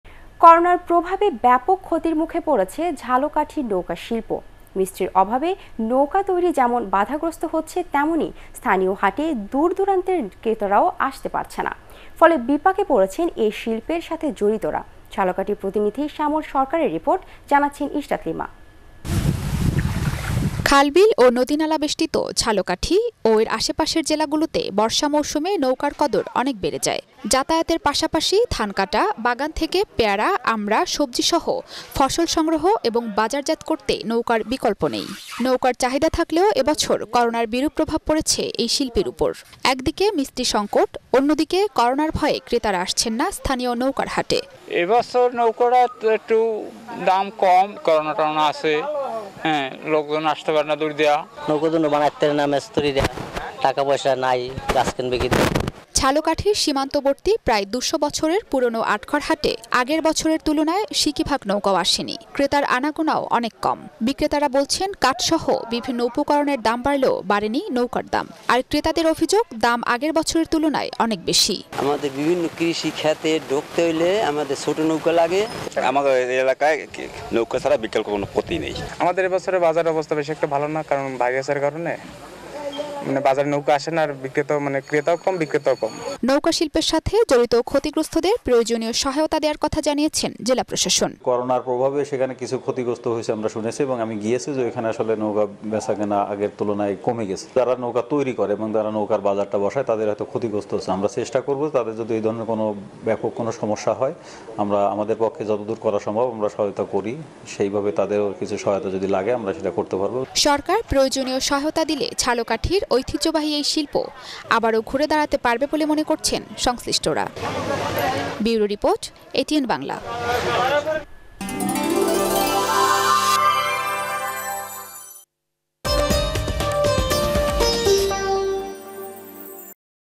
করোনার প্রভাবে ব্যাপক ক্ষতির মুখে পড়েছে ঝালকাঠির নৌকা শিল্প। ATN Bangla News
করোনার প্রভাবে ব্যাপক ক্ষতির মুখে পড়েছে ঝালকাঠির নৌকা শিল্প। মিস্ত্রীর অভাবে নৌকা তৈরি যেমন বাঁধাগ্রস্থ হচ্ছে তেমনি স্থানীয় হাটে দূর-দূরান্তের ক্রেতারাও আসতে পারছেন না। ফলে বিপাকে পড়েছেন এ শিল্পের সাথে জড়িতরা।